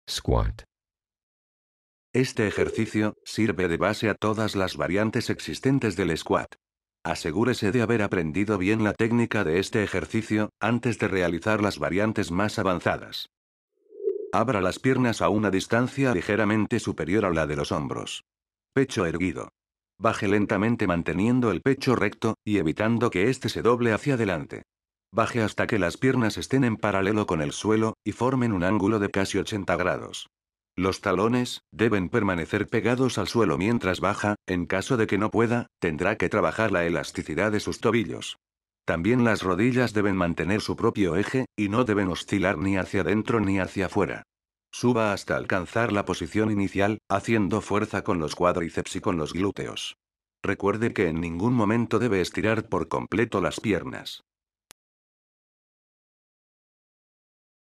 Ejercicio "El Squat o Sentadillas" (flexion de rodillas) para piernas y los gluteos
Squat ejercicios y ejercicio  fitness para  cuadriceps, desarrollar los gluteos